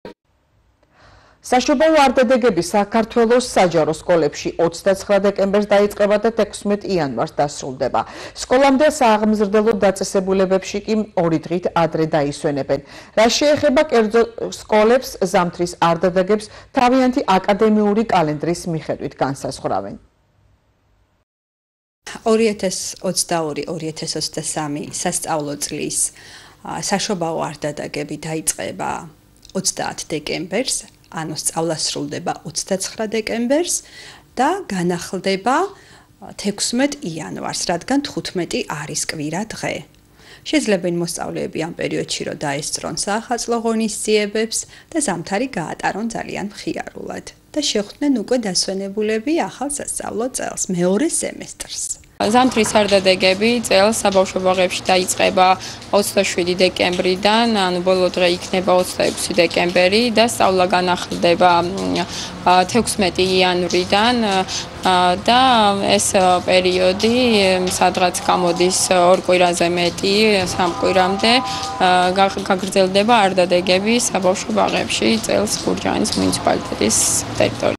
როდის იწყება საშობაო არდადეგები სკოლებსა და ბაღებში
საშობაო არდადეგები საქართველოს საჯარო სკოლებში 29 დეკემბერს დაიწყება და 16 იანვარს დასრულდება. სკოლამდელ სააღმზრდელო დაწესებულებებში კი 2 დღით ადრე დაისვენებენ. რას შეეხება კერძო სკოლებს, ზამთრის არდადეგებს თავიანთი აკადემიური კალენდრის მიხედვით განსაზღვრავენ.